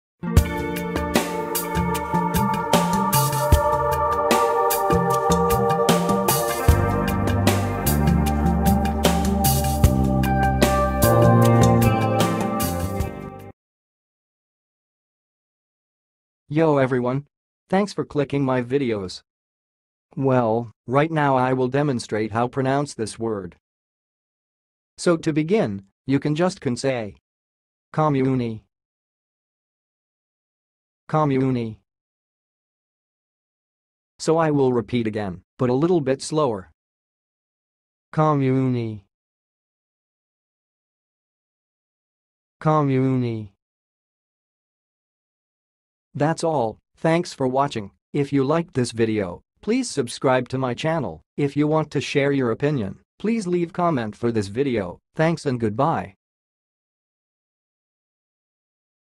KOMUUNI - HOW TO PRONOUNCE KOMUUNI?
KOMUUNI - Say it right: Pronouncing Komuuni in 2023

How do you pronounce "komuuni"?
What is the correct emphasis when saying "komuuni"?
Are there any alternative pronunciations for "komuuni"?
Definition:
Komuuni is a Finnish word that refers to a small township or municipality.
It is typically used to describe a local administrative unit in rural areas of Finland.
These komuunis often have a small population and may encompass several villages or towns.
Pronunciation:
To pronounce "komuuni", break it down as koh-moo-nee with emphasis on the second syllable.
Test yourself by speaking the following examples:
1. I live in a komuuni in the Finnish countryside
2. The komuuni organizes a summer festival every year
3. The local school in the komuuni is very small
Hope it will help you to answer on the following questions:
1. What are some examples of komuunis in Finland?
2. How does a komuuni differ from a city?
3. What services are provided by the komuuni government?